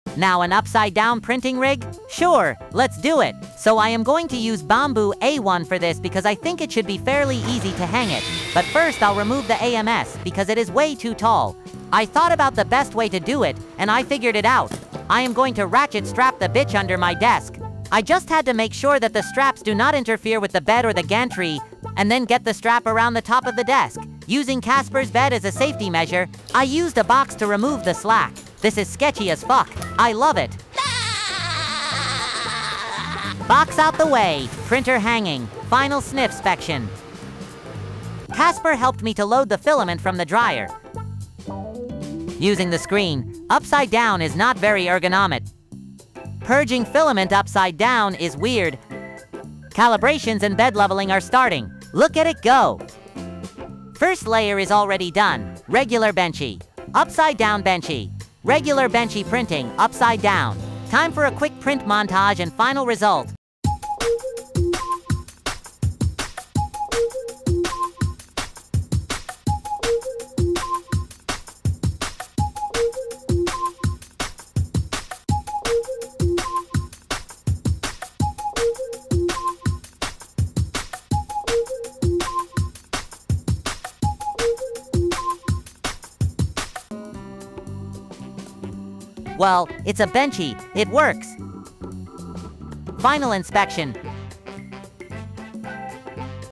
I hung my 3D Printer upside down to see what happens...
$7.79 Per roll when you buy 20

$8.08 Per roll when you buy 12

$9.40 Per roll when you buy 6 -